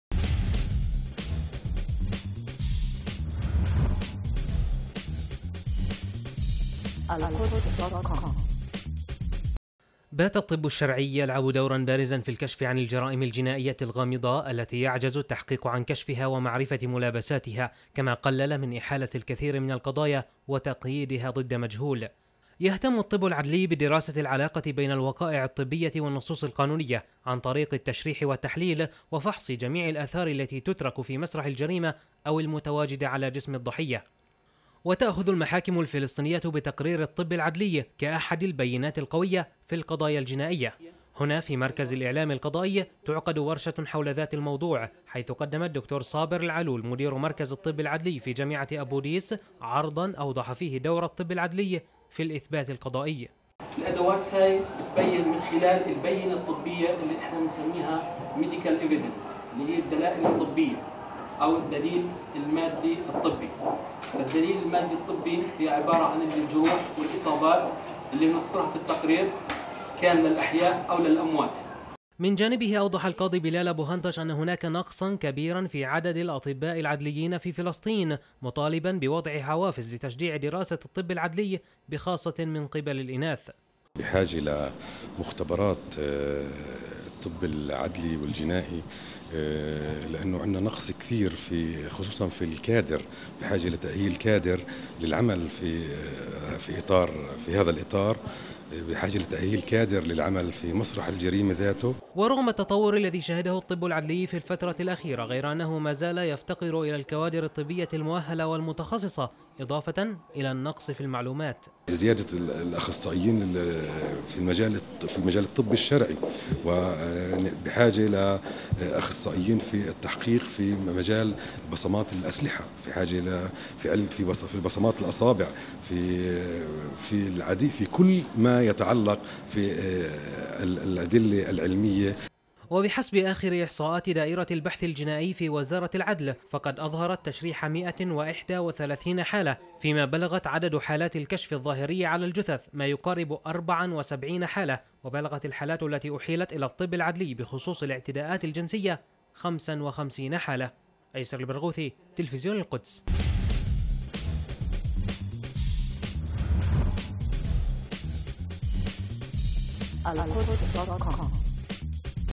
التشريح.. الدليل العلمي
محاضرة تعريفية حول دور الطب الشرعي في البحث الجنائي